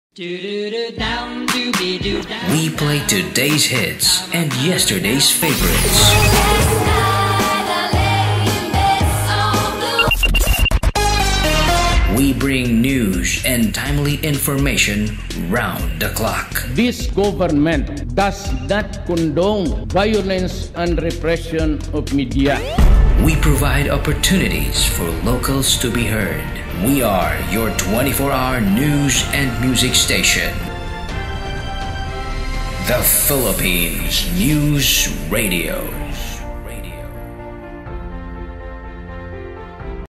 Muews radio Philippines
Latest plug after every station ID. start of Broadcast September 2016 Muews Radio Tarlac